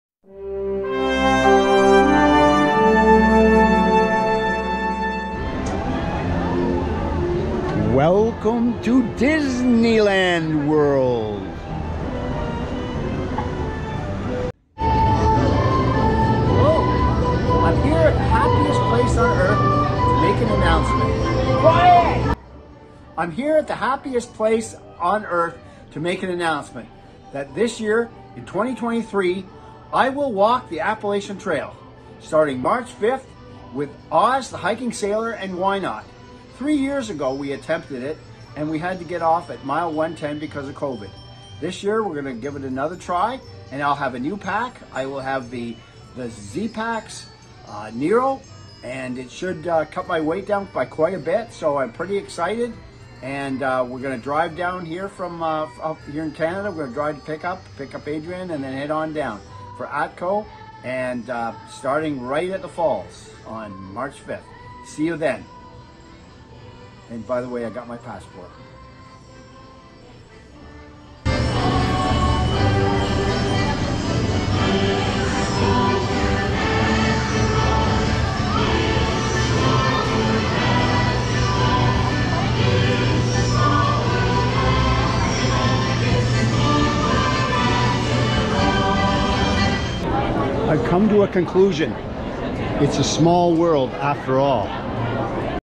Appalachian Trail Announcement 2023
I will be starting the Appalachian Trail for a Fourth Voyage in March 2023.
The last time we had to get off the trail in 2020 due to the covid outbreak. Once again I will be with  0z The Hiking Sailor @0zthehikingsailor and Whynot @whynotadventures5314 . We drive down from Canada attend ATKO (Appalachian Trail Kick Off) and step off from the Amicalola Falls on March 5th. I'm excited to try out my new Zpacks Nero pack and seeing how little weight I can get away with.